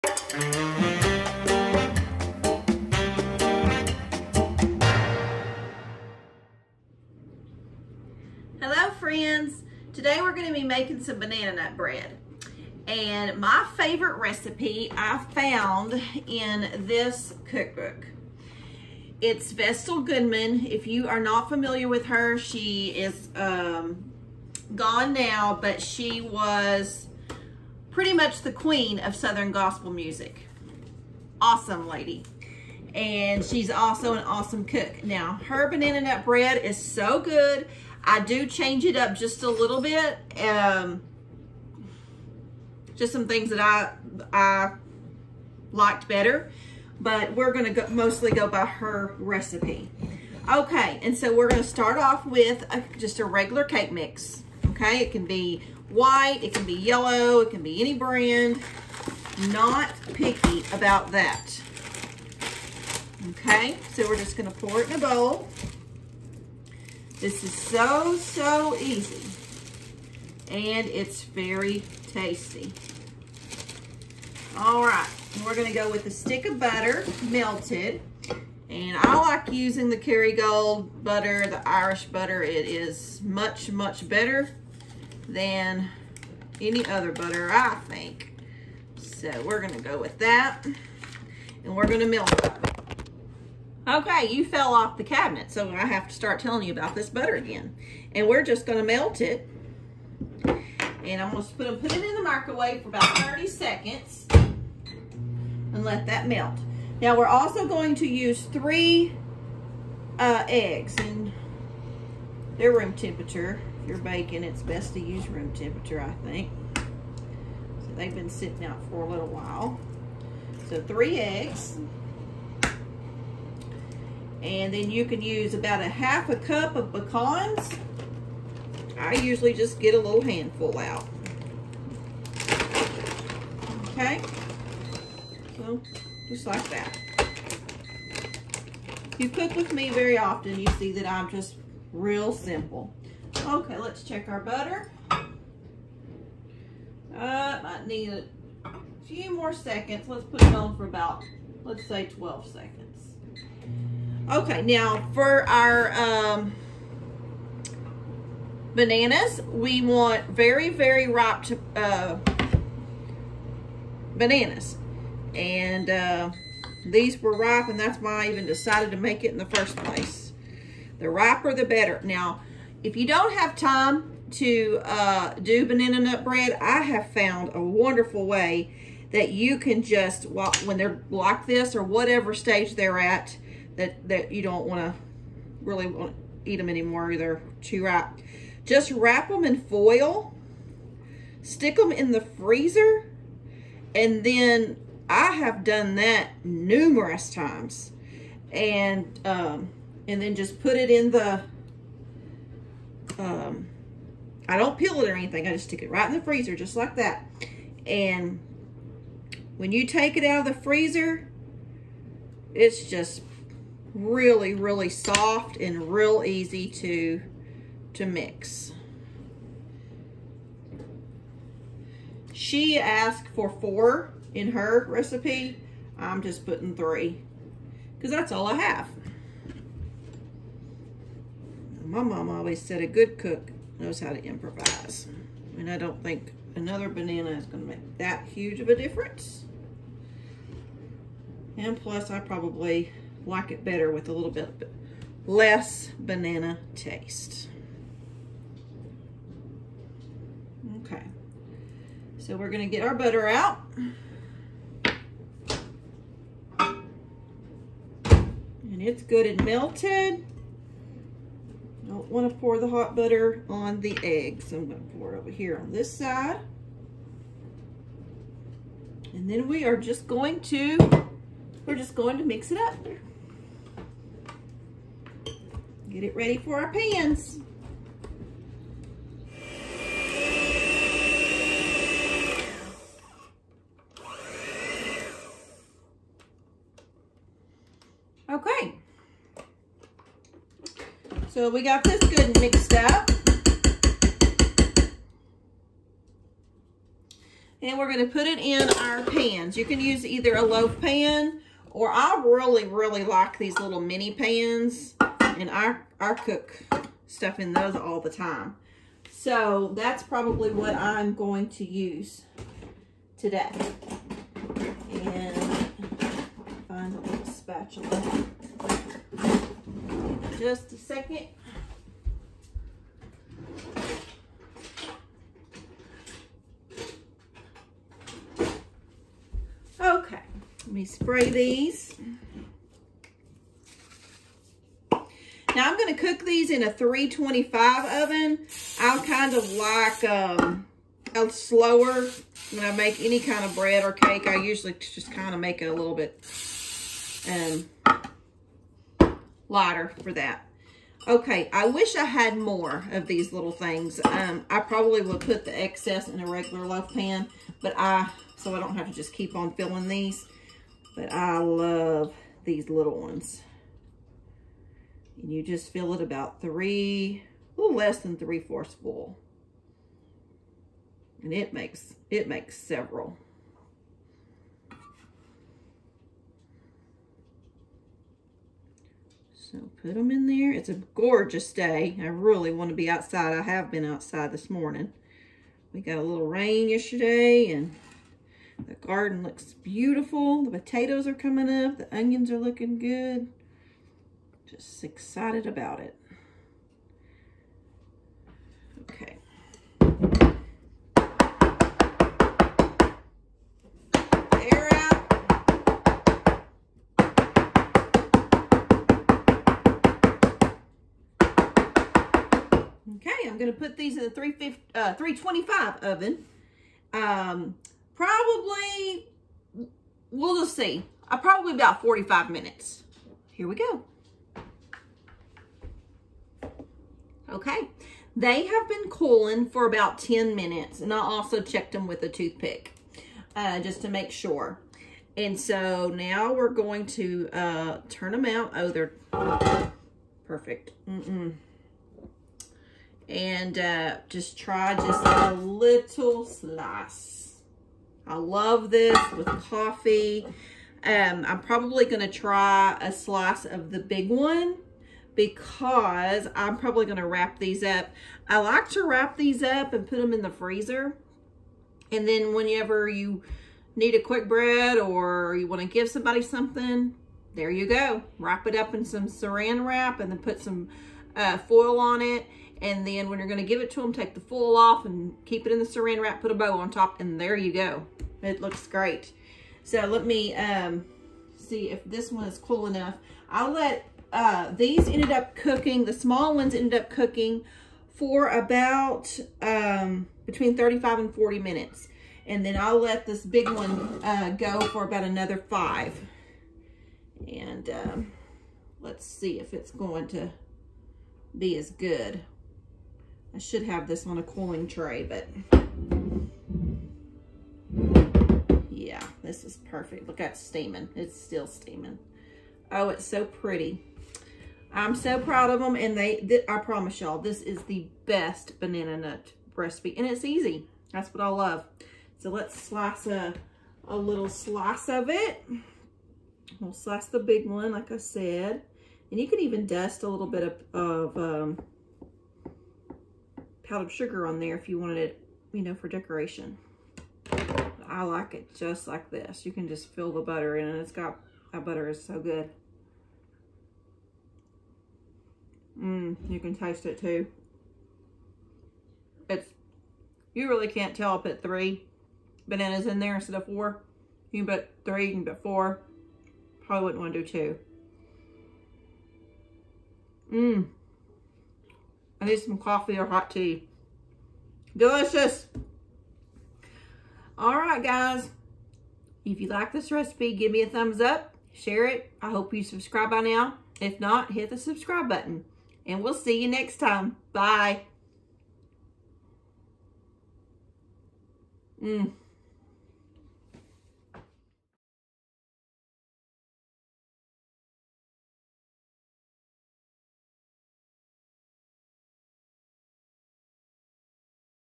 BANANA NUT BREAD- Vestal Goodman’s RECIPE
This is the best banana nut recipe ever. Moist. Buttery. Easy.

Boots and Bonnets
Sunset, Texas 76270